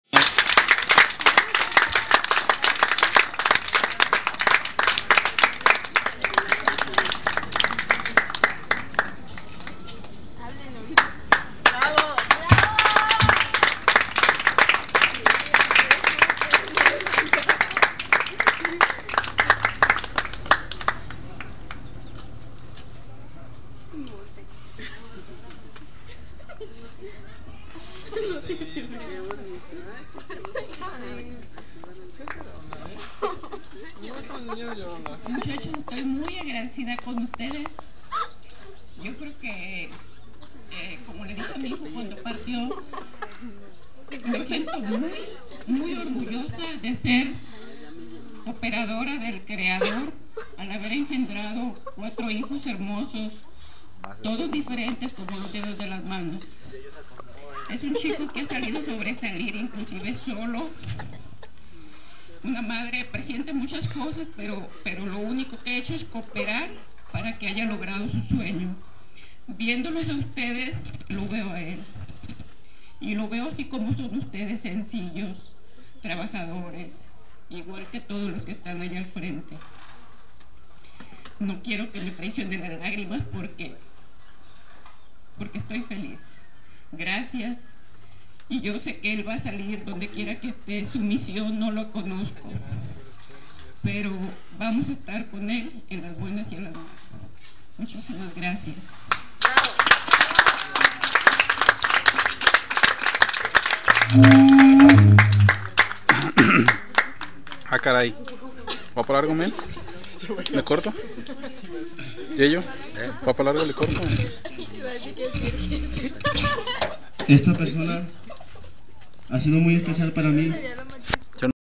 ANIVERSARIO CORO SANTA CRUZ
ESTO ES ESPECIAL PARA UN AMIGO QUE LLEVO EN EL CORAZÓN NACHITO CALVARIO HASTA PERU VA ESTO CON TODO CARIÑO